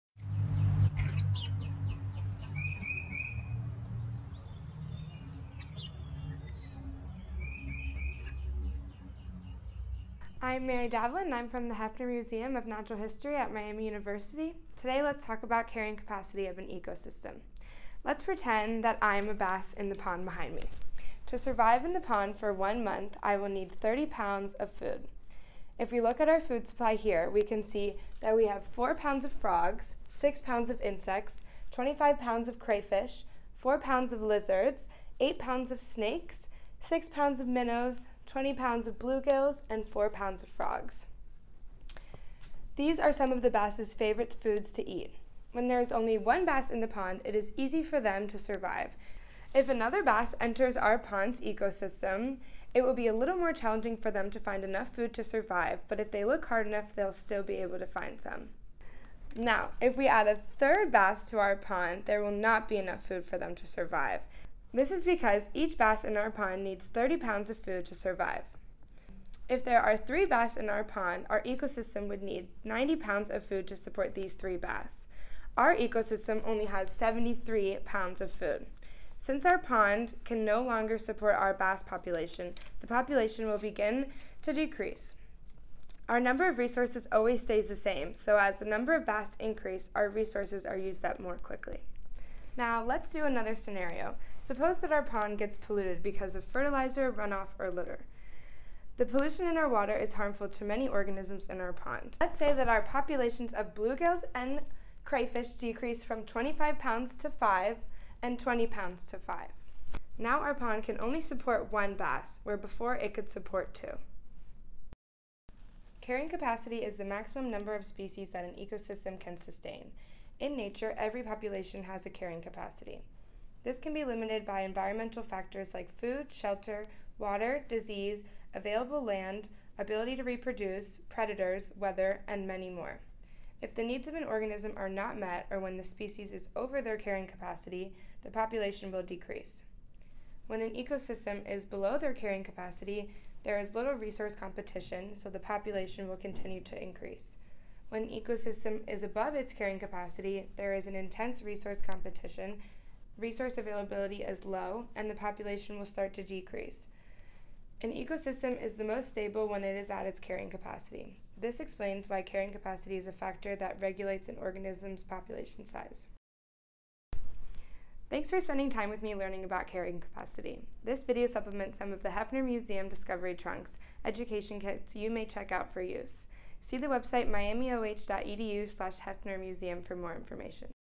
A Lesson on Carrying Capacity
Mary Davlin
Created for BIO 320 at Miami University “Middle Childhood Environmental Educator Certification” Spring 2021

This video introduces students to the idea of carrying capacity. As students observe the population of bass in a pond grow, they learn that their resources remain the same. When the pond gets polluted, students witness resources be removed from the ecosystem. The availability of resources in our scenario allows students to understand what determines the carrying capacity of an ecosystem, or the number of organisms an ecosystem can sustain.
This video supplements the Pond Life Discovery Trunk provided by the Hefner Museum of Natural History.
Target Grade Level: 6-8